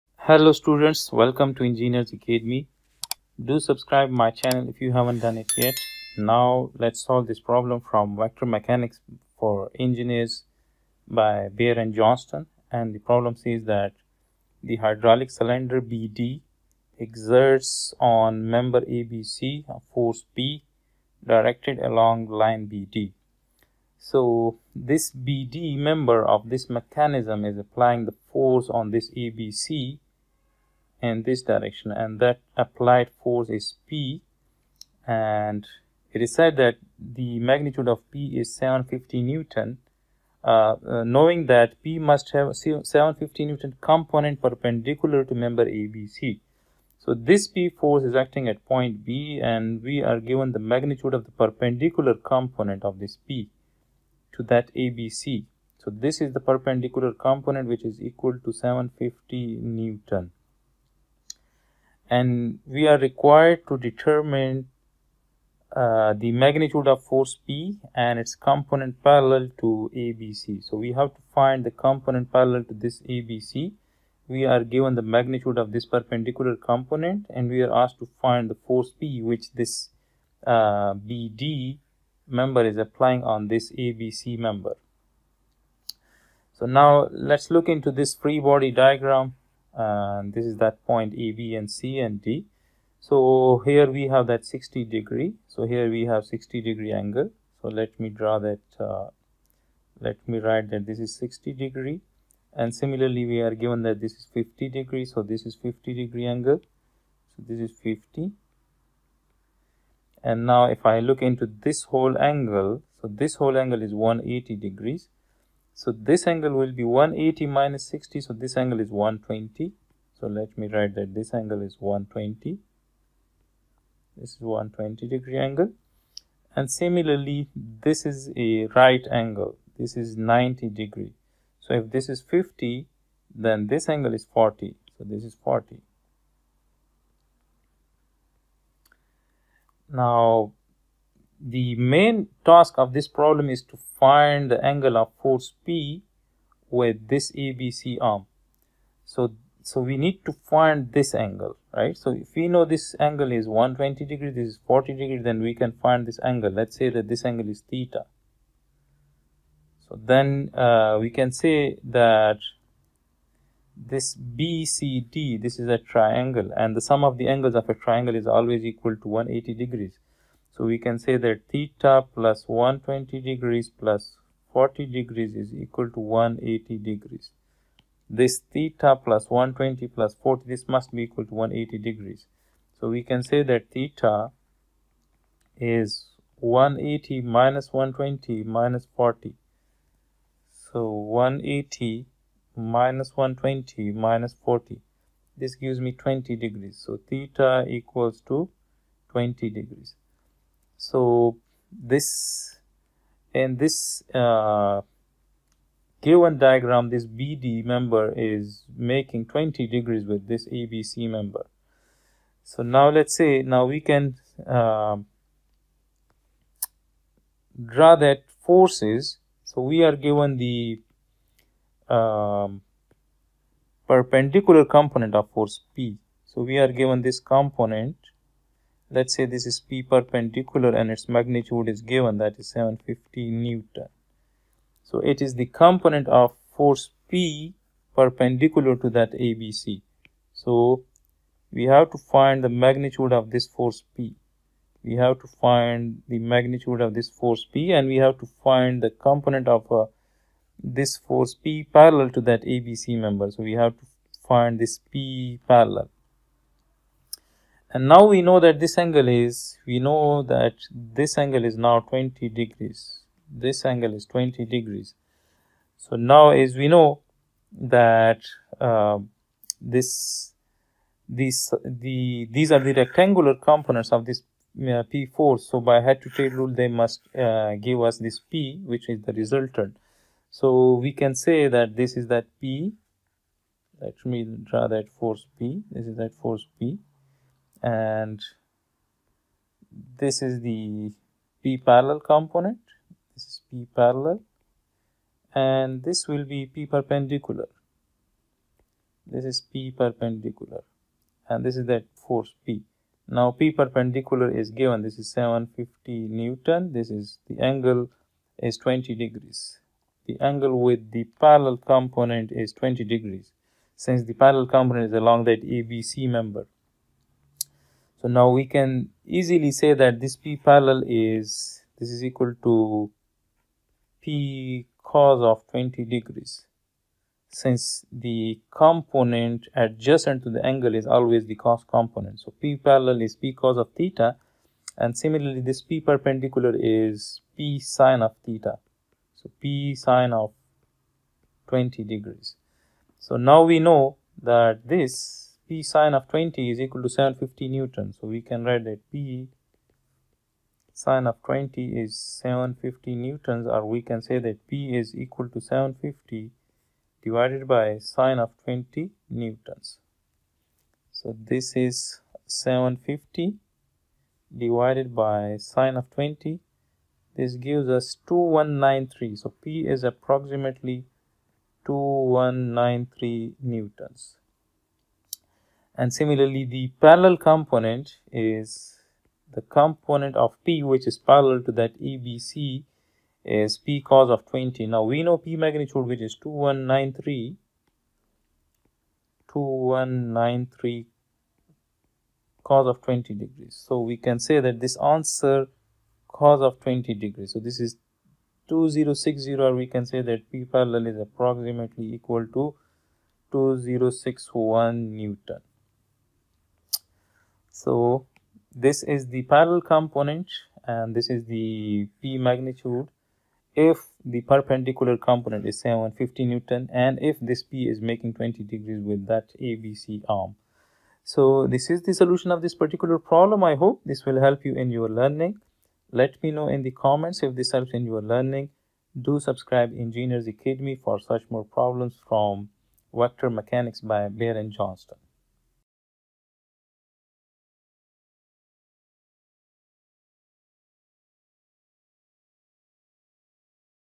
2.25 The hydraulic cylinder BD exerts on member ABC a force P | Beer & Johnston | Engineers Academy
Vector mechanics for engineers by Beer and Johnston solution

2.25 The hydraulic cylinder BD exerts on member ABC a force P directed along line BD, Knowing that P must have a 750-N component perpendicular to member ABC, determine (a) the magnitude of the force P, (b) its  component parallel to ABC.

Law of Cosines
Parallelograms Law
Vector Addition

2.25 The hydraulic cylinder BD exerts on member ABC a force P  | Beer & Johnston | Engineers Academy